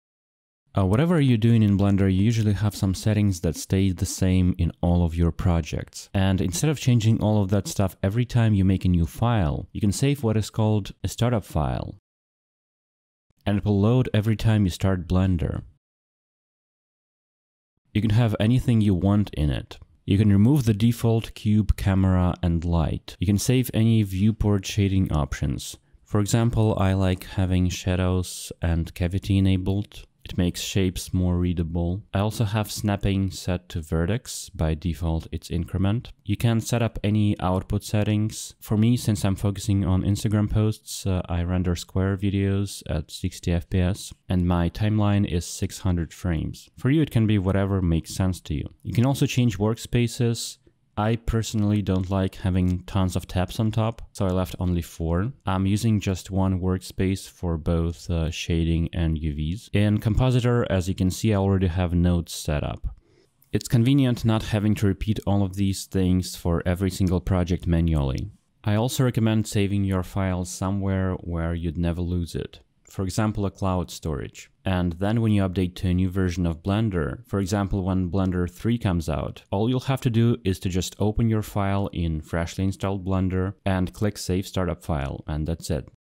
Startup File in Blender - Quick Tip
In this quick tip I show how to change some blender's default startup settings with startup file